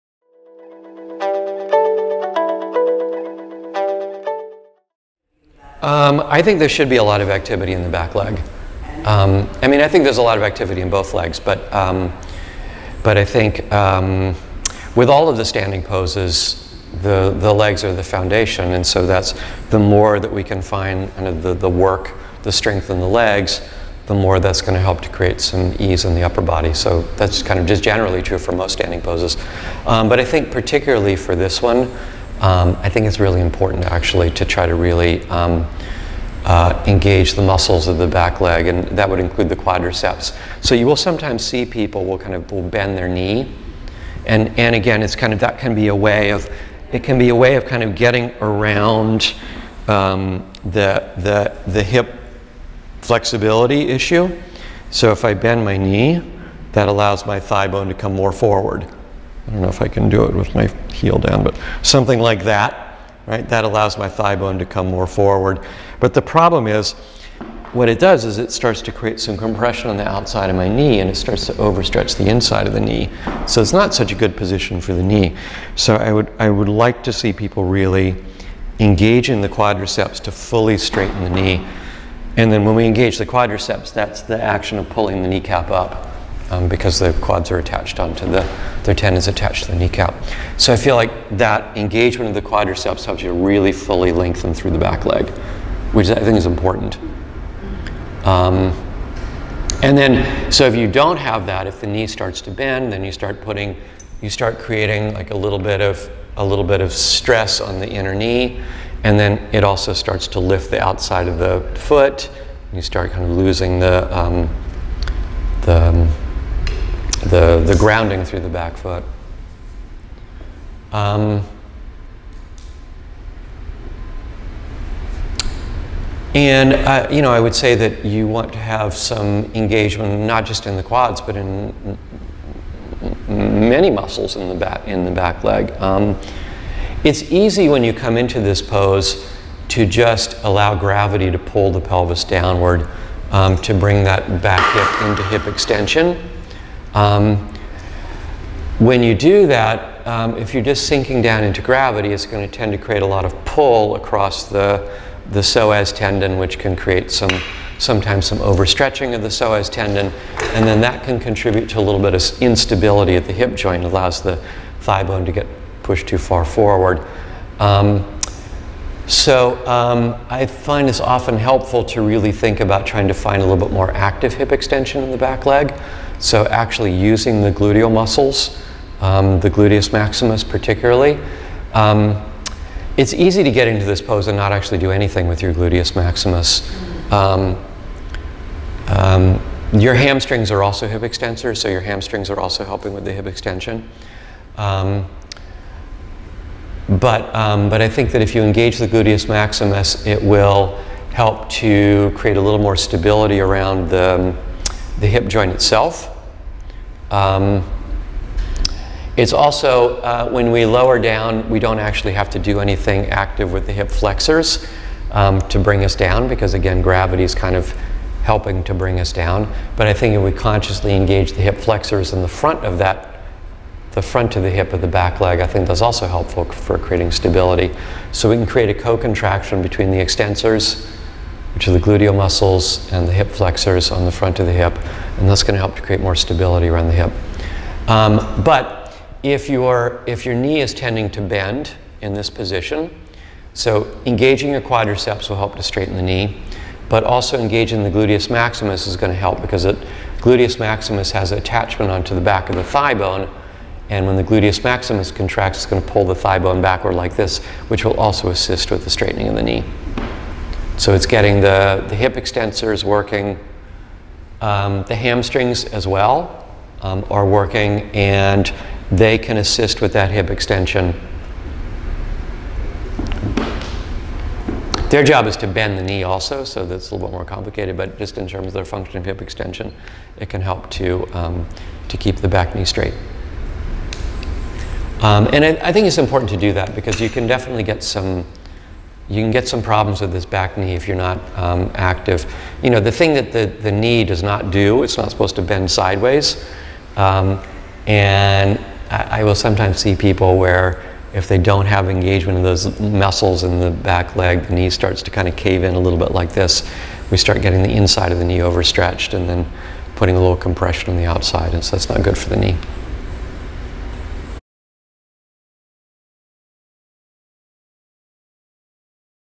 Should you engage the back leg in warrior 1?
In this video from an anatomy workshop about Warrior 1 at the Shala in NYC, I answer a question from a student about how much should you engage the quadriceps muscles of back leg in Warrior 1.